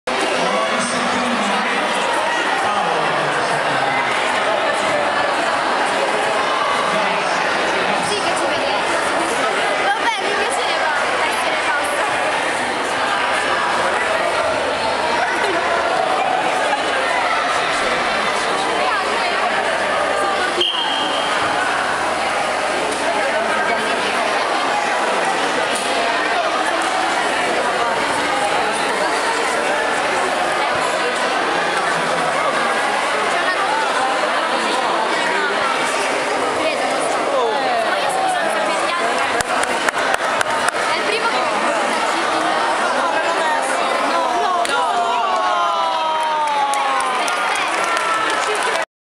SOS 2018
XII Festival della Robotica educativa
18 maggio 2018
Gara: S.O.S. Squadra di soccorso